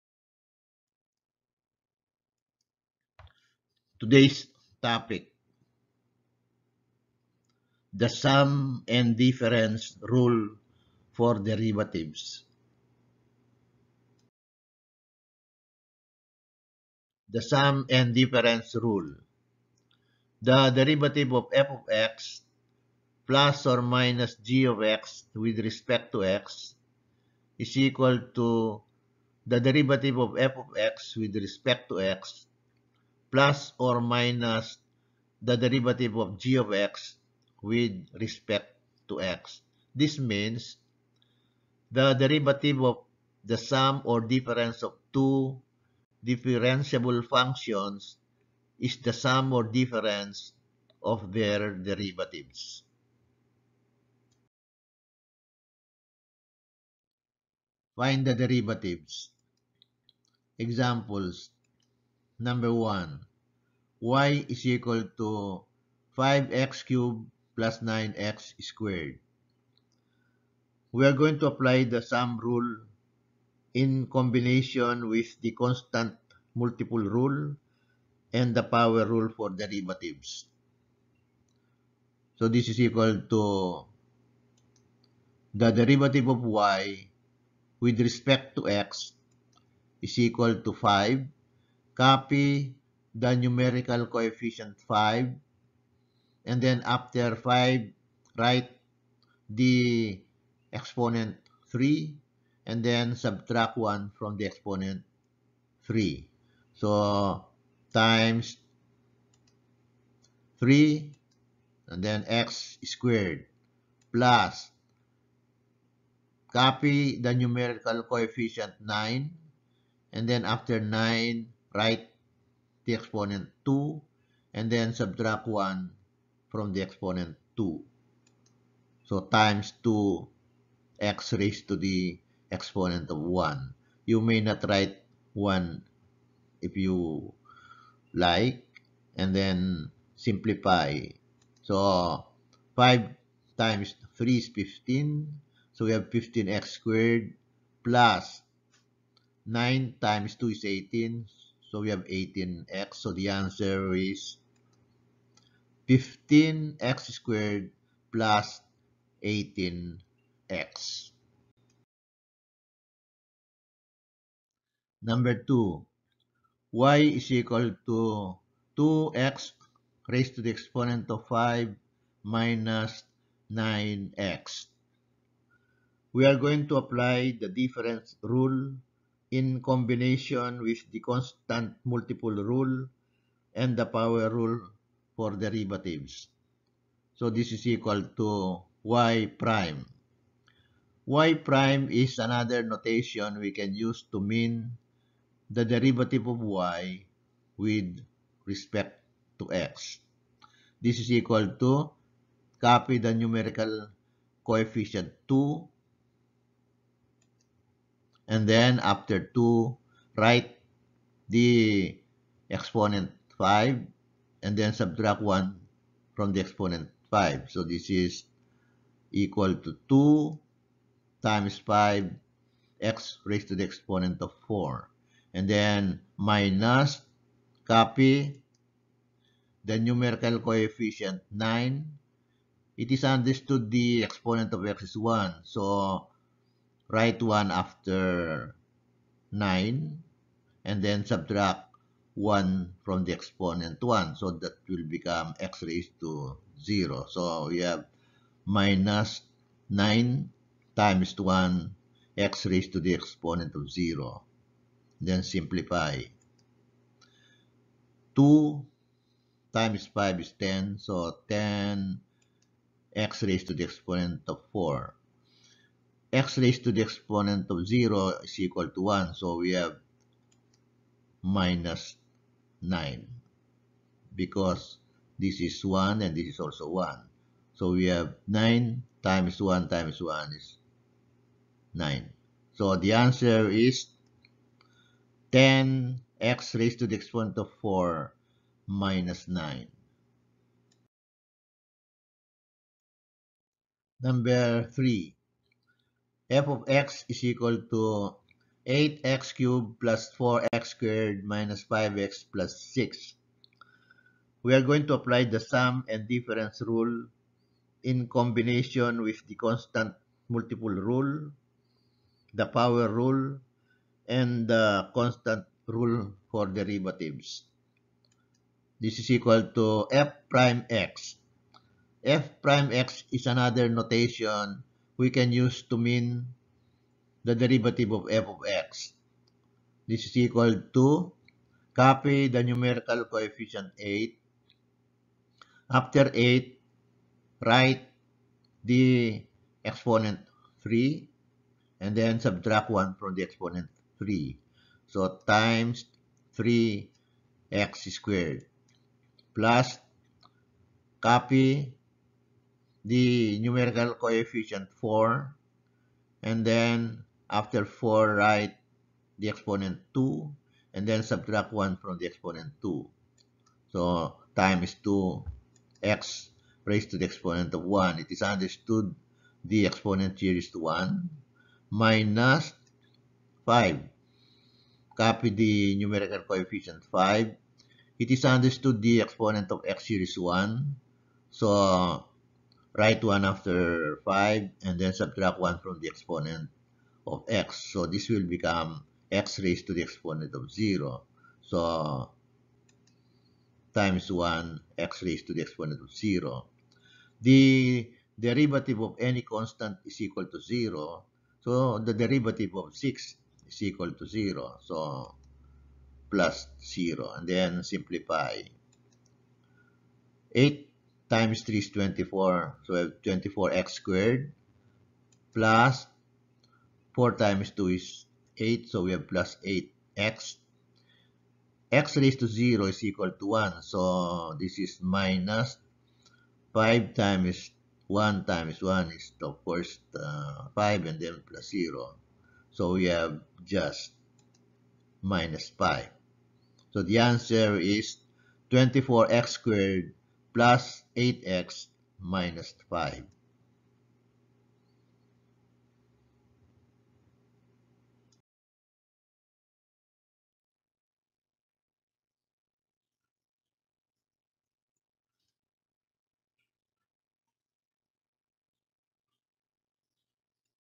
The Sum and Difference Rule for Derivatives | Calculus | Math Video Central
How to apply the sum and difference rule for derivatives is illustrated in this video.